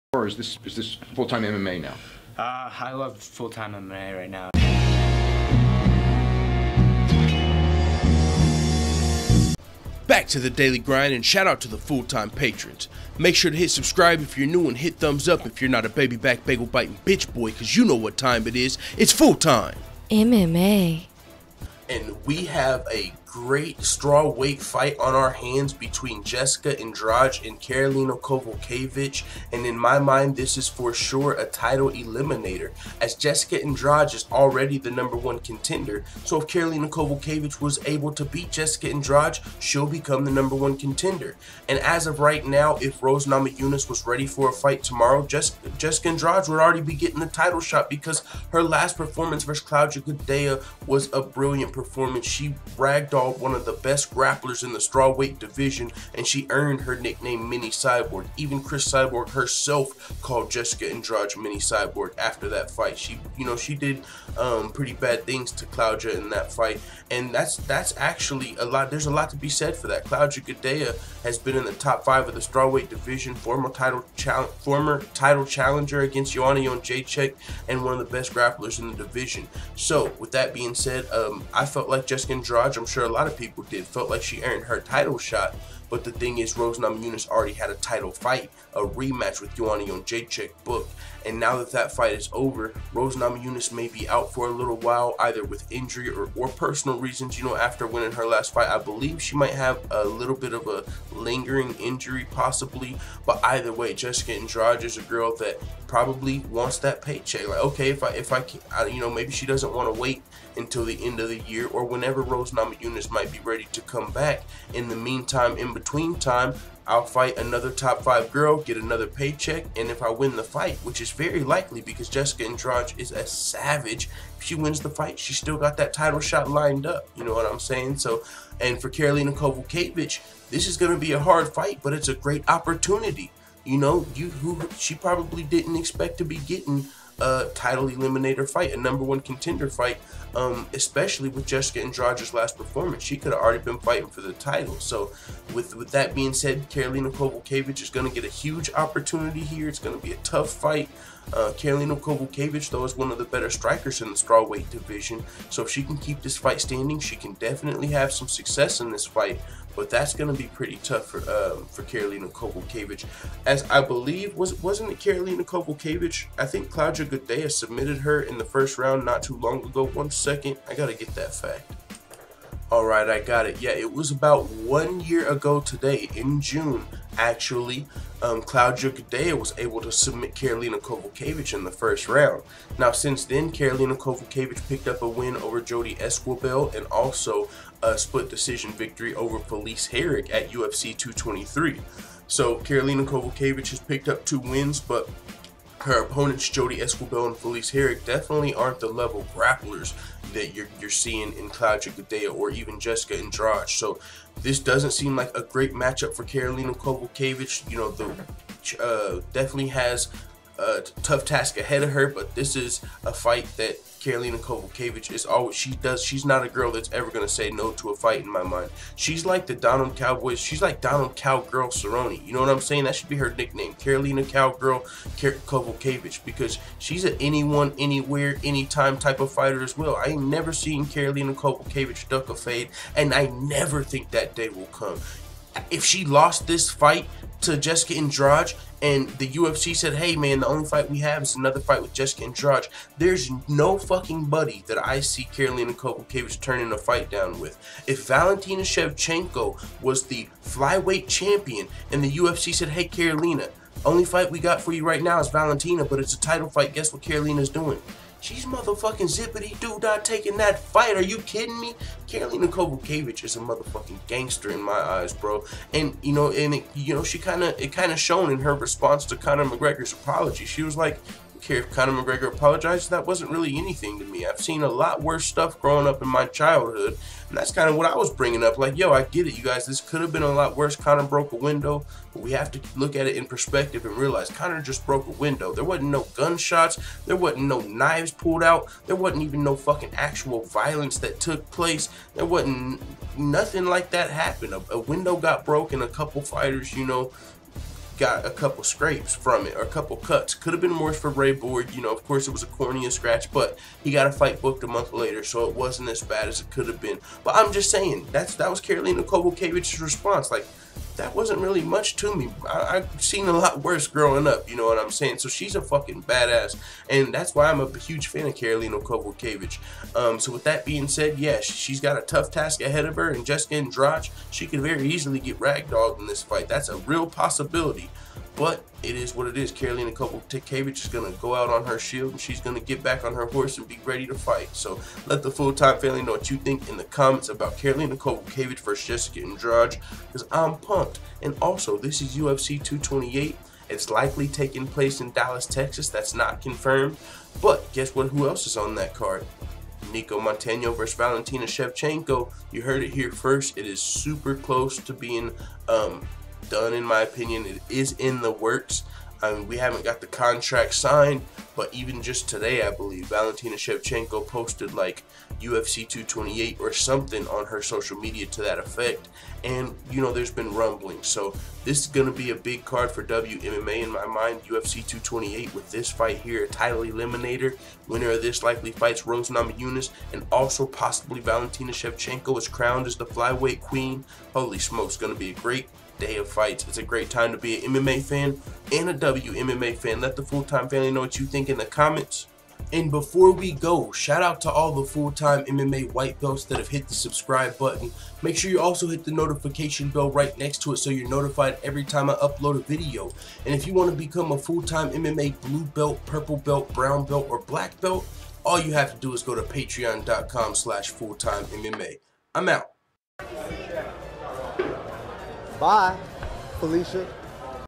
Jessica Andrade vs Karolina Kowalkiewicz in the Works for UFC 228 in Dallas
▶ For more FREE videos like this plus UFC Predictions/Breakdowns, Betting Advice and more check out